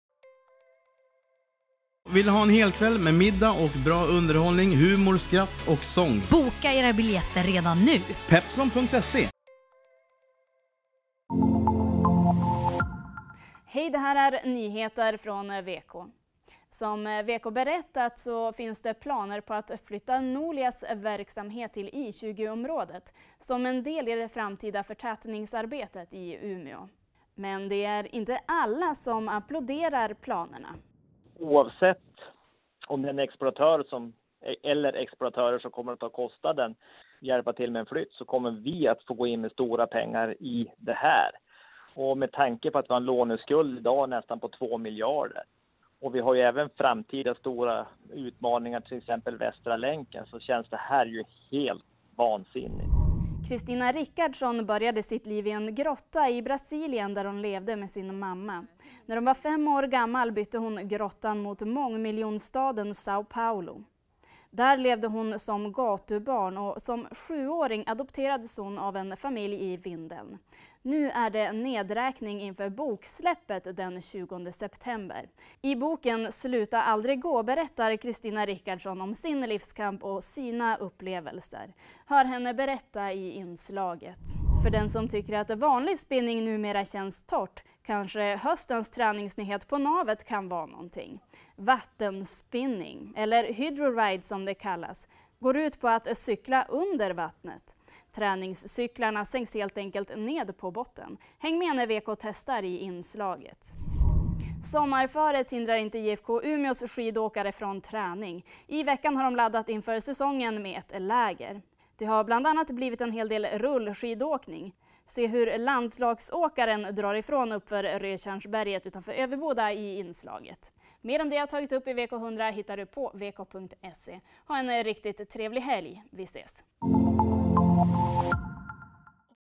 VK100: "Vansinnigt att flytta Nolia"
Det finns planer på att flytta Nolias verksamhet till I20-området i Umeå. Men det är inte alla som applåderar planerna. Se dagens VK100!